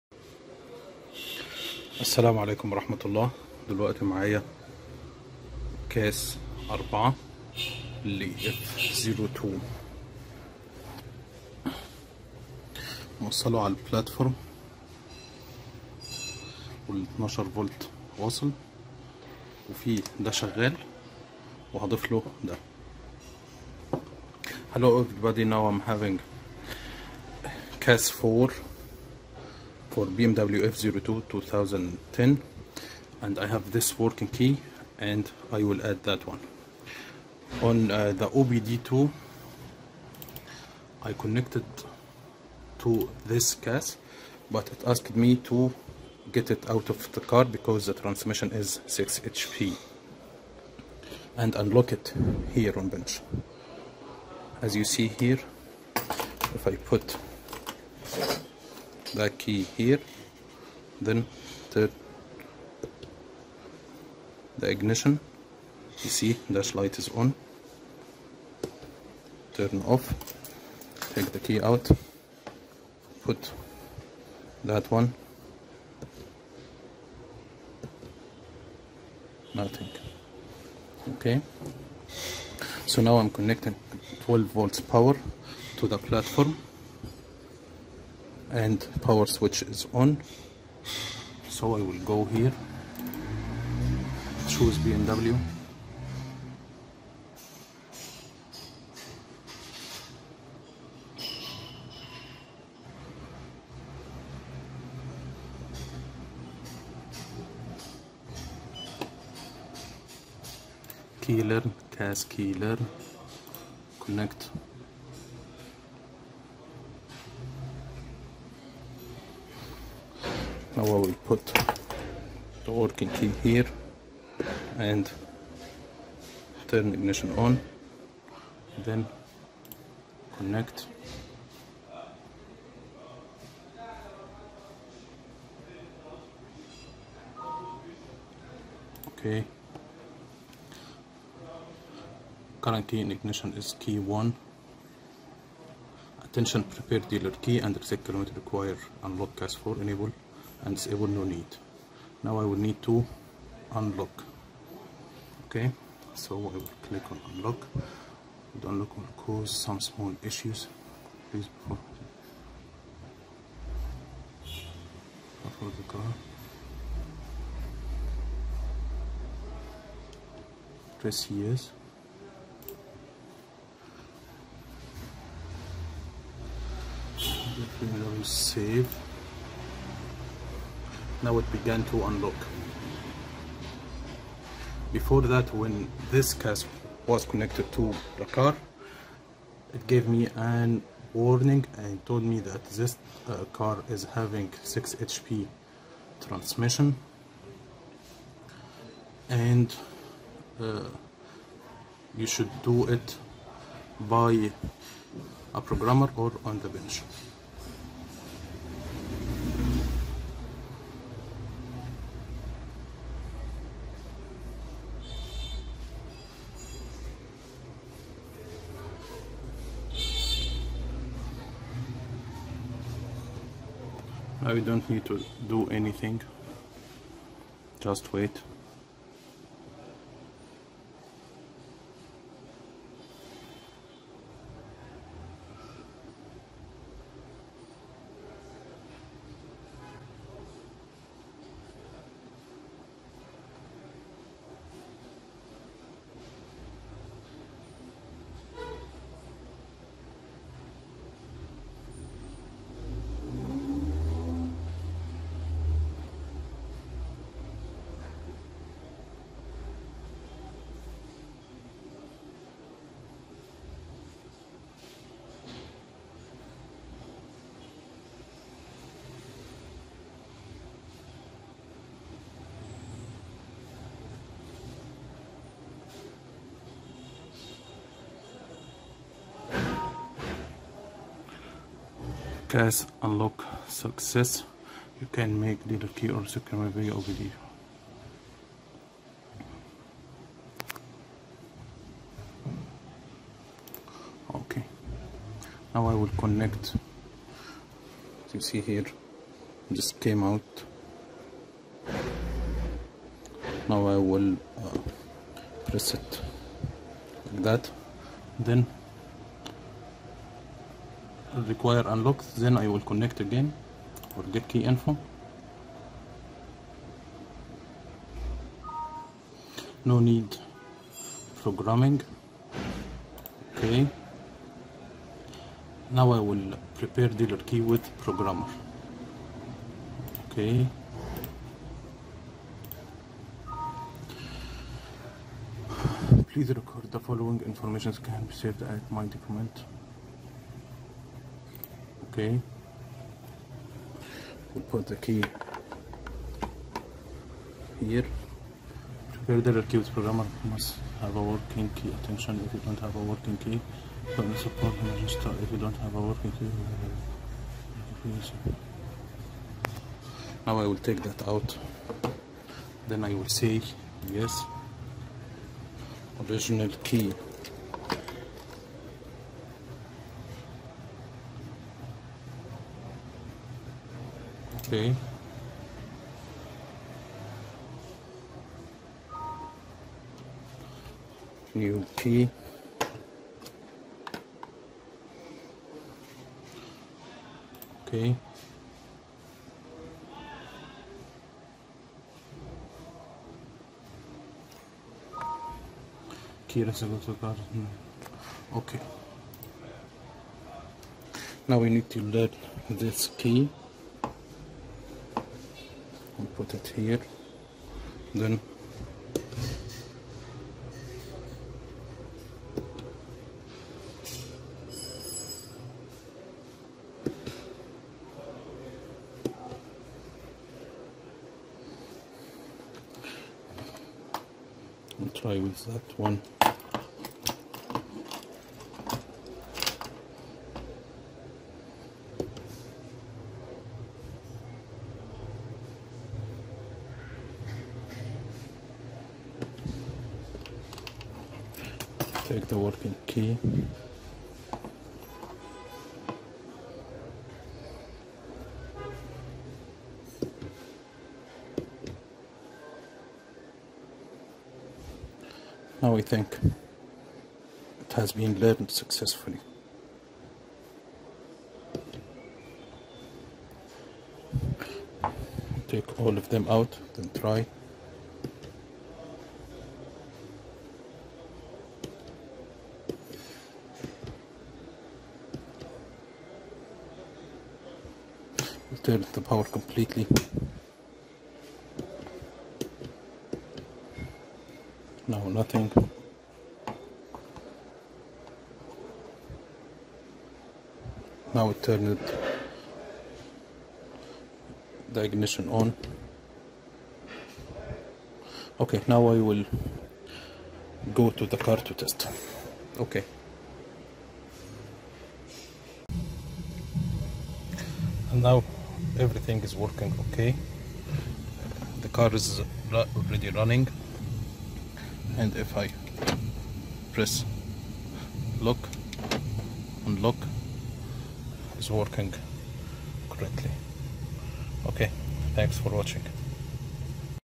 Cas4 Unlocking & Newer Shape Key Progamming By Vvdi2
Bmw F02 750 Li Adding Spare Key On Bench By Vvdi2 ... Using Cas4 Bench Platform